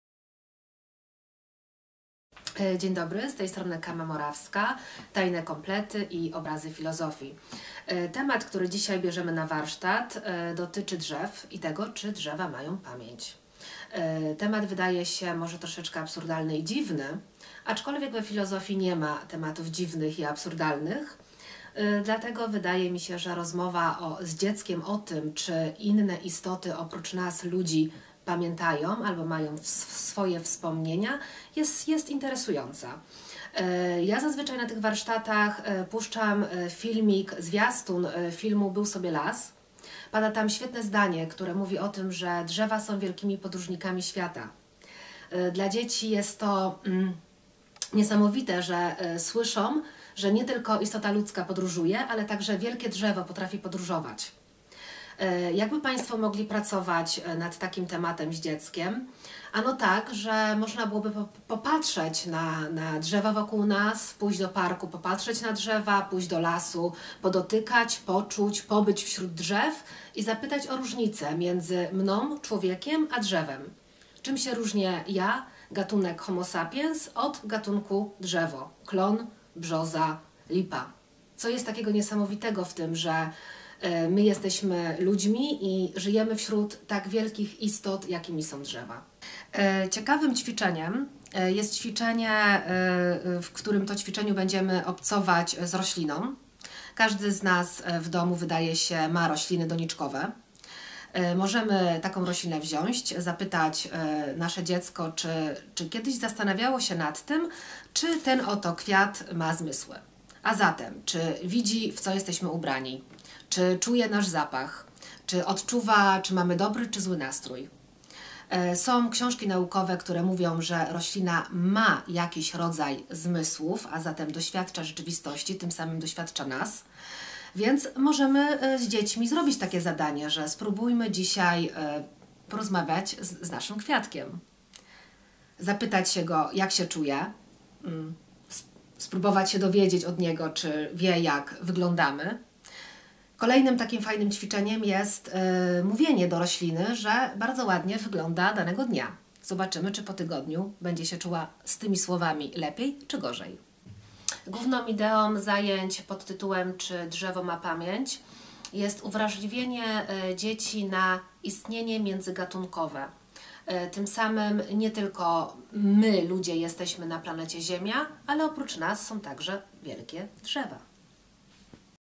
Obrazy Filozofii: Czy drzewo ma pamięć?
Obrazy Filozofii to cykl warsztatów dla najmłodszych prowadzony przez Kamę Morawską.

Dzieci, co niejednokrotnie podkreślają wybitni filozofowie, mają naturalną skłonność do zadawania pytań o charakterze filozoficznym, jak również krytycznym. Potencjał filozoficzny dziecka – jego zdziwienie światem, wątpienie, zapytywanie, zdumienie, konfuzja – nie może zostać ani marginalizowany, ani – tym bardziej – zaprzepaszczony. Warsztaty filozoficzne dla dzieci mają odpowiadać na powyższe potrzeby, jak również modelować, stymulować i twórczo rozwijać myślenie filozoficzne i filozoficzne obrazowanie świata. Celem do osiągnięcia jest autonomia myślenia małego człowieka. Owocem filozoficznego dociekania winna być sama intelektualna praca dziecka, ale i poszerzenie horyzontów, poznanie siebie i poznanie nowego oraz ekspresja siebie i konfrontacja z ekspresją innych.

Dofinansowano ze środków Ministerstwa Kultury i Dziedzictwa Narodowego pochodzących z Funduszu Promocji Kultury.